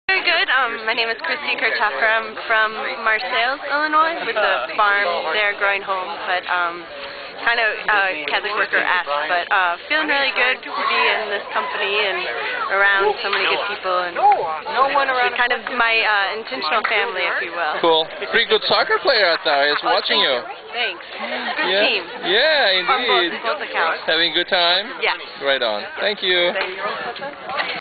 Interviews performed at the Sugar Creek Catholic Worker Retreat on 21 Sep 2008